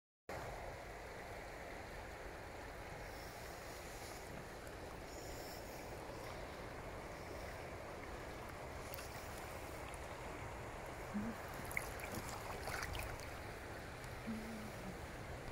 Grayling catch and release
Grayling from river Vah, Slovakia